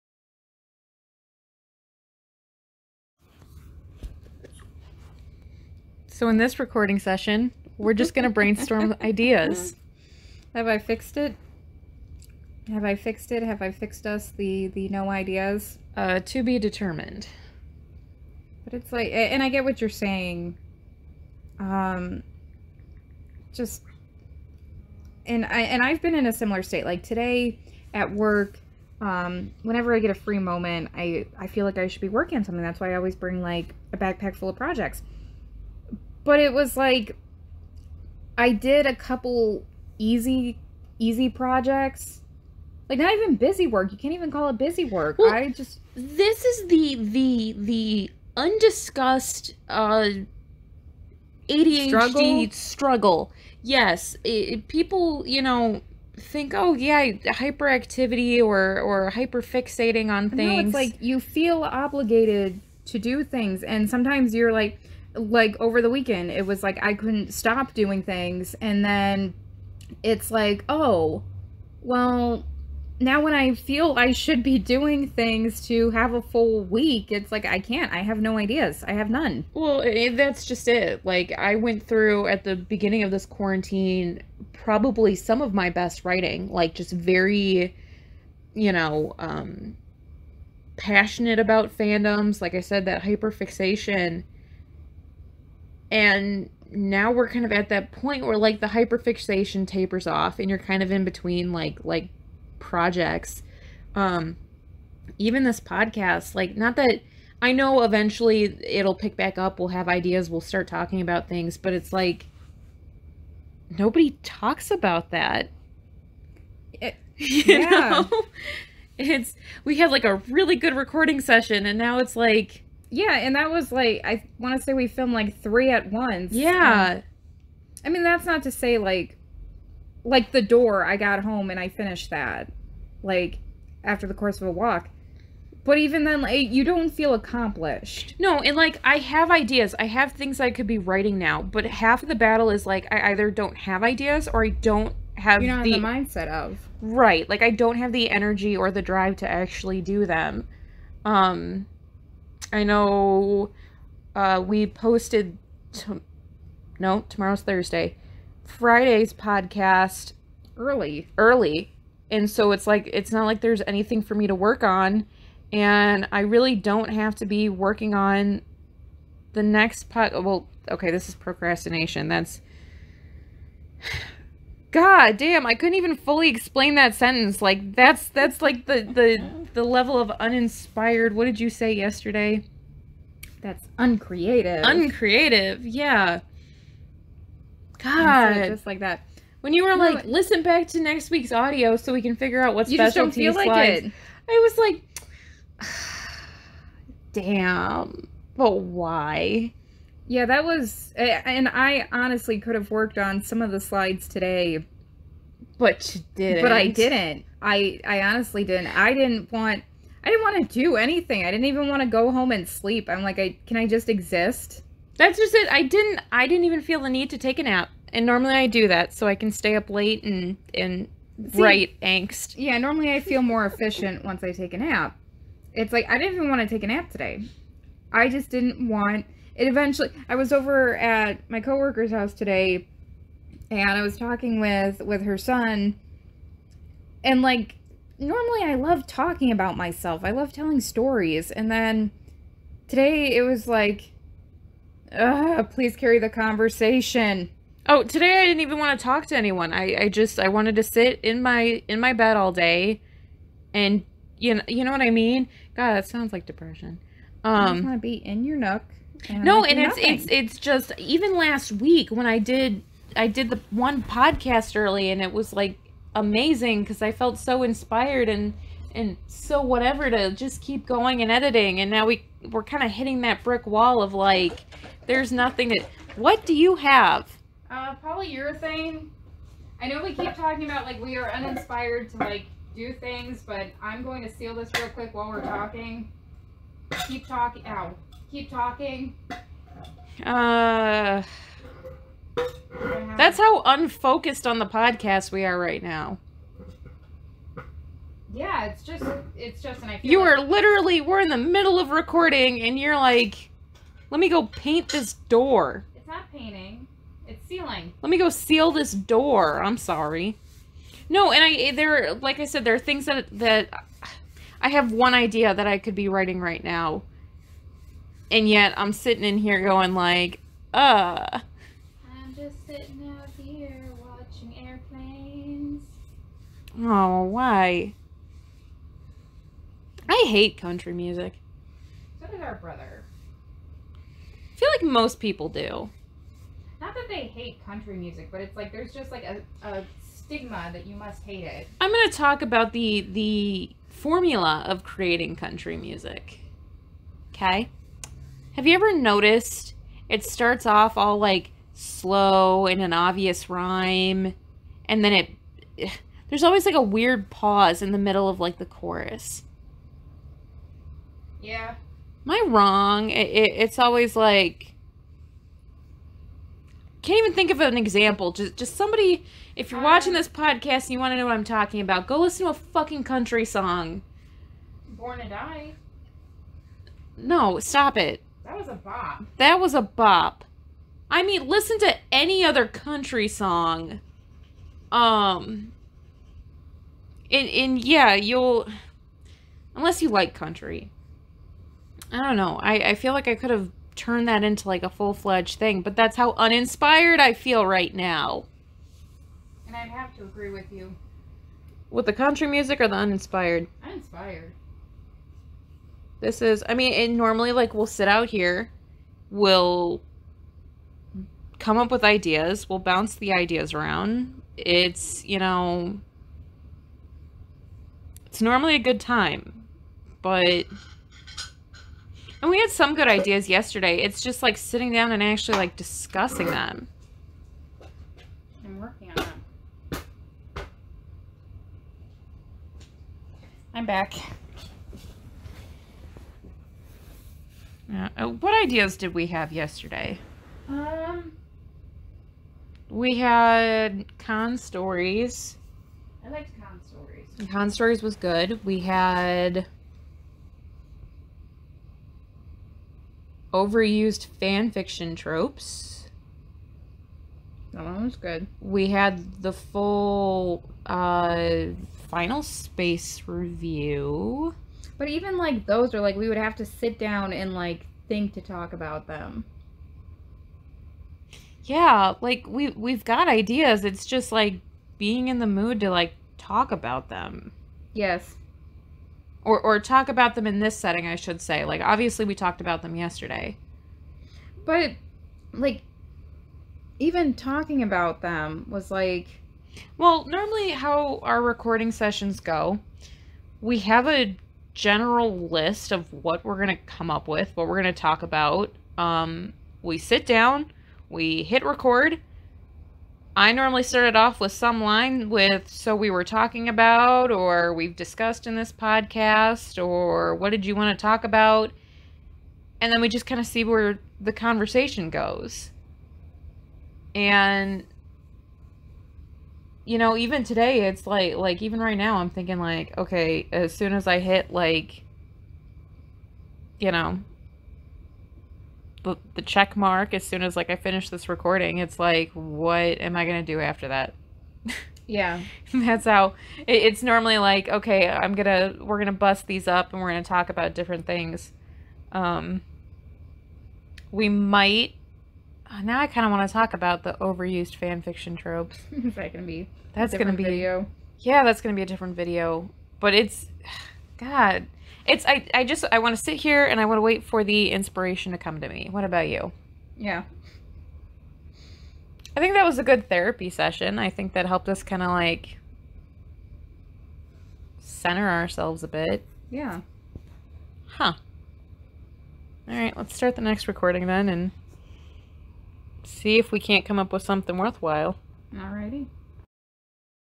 Sh*t Tea And ADHD 12: Brainstorming
A podcast made up of our car conversations, driven all over the place by our ADHD, and edited down by Emily.

We figured if we were going to be having random conversations, we might as well make something productive out of them.

Images courtesy of Hntrgurl13 and Lauren